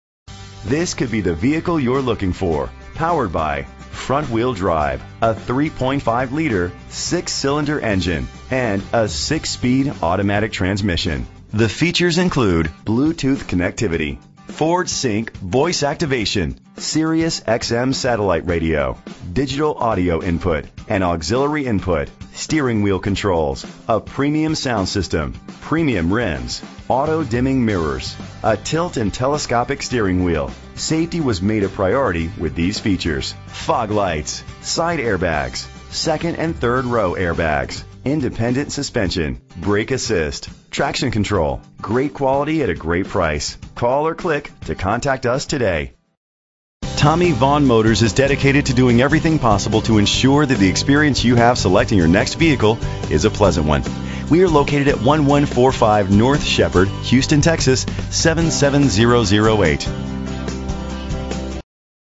2015 Ford Explorer - Houston TX
Phone:
Year: 2015
Make: Ford
Model: Explorer
Trim: XLT
Engine: 3.5 liter 6 cylinder 24 valve
Transmission: 6-Speed Automatic
Color: Ruby Red Metallic Tinted Clearcoat
Mileage: 0
Address:
1145 Shepherd Drive